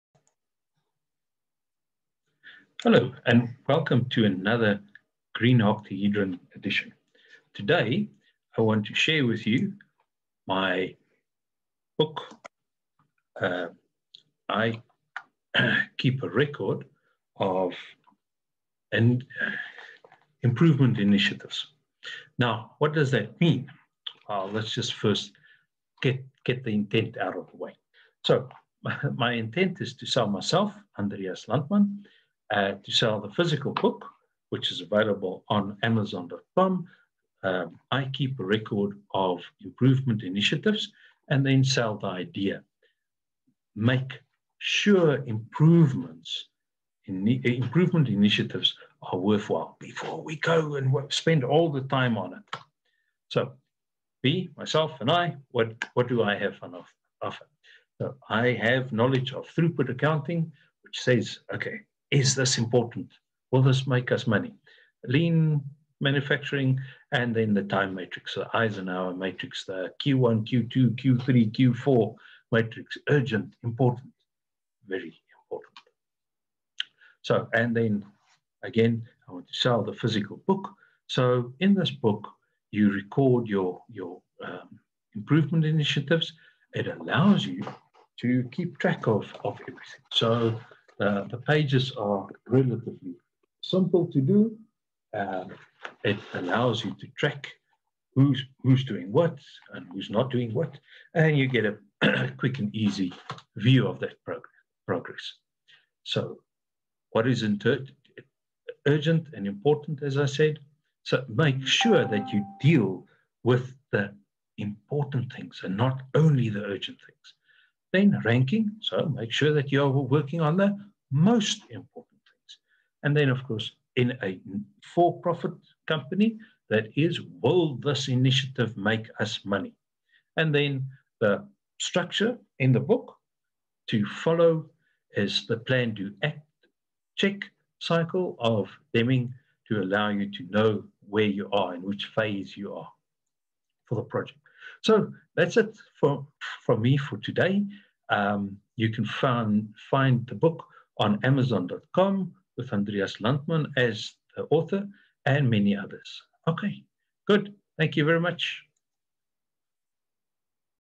GOC Improvement Initiatives 20210409
In this Green Octahedron Edition Andreas shares the idea that any improvement initiative must be important and have a measurable impact on the company's goal.